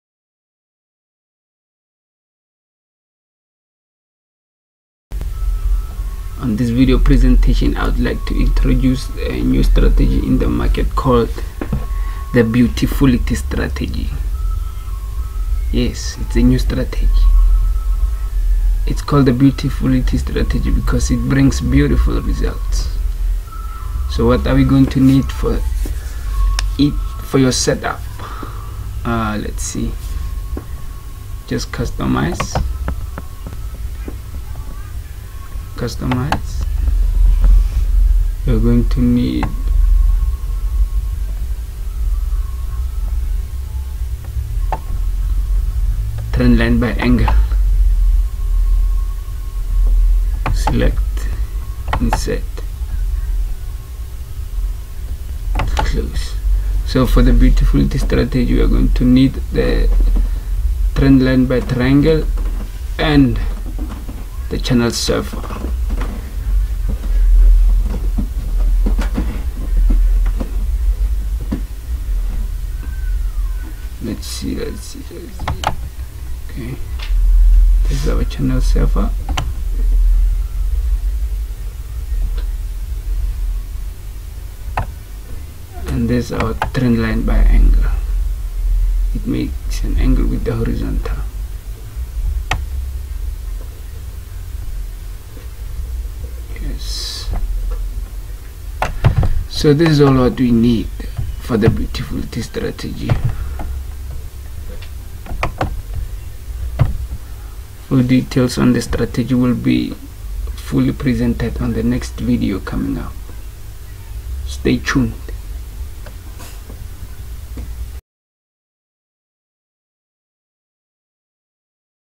Introduction of the best forex strategy
This is the best 90% Strategy of Forex we came up with and it is 90% accurate. If you need to know more about it, comment on a comments below.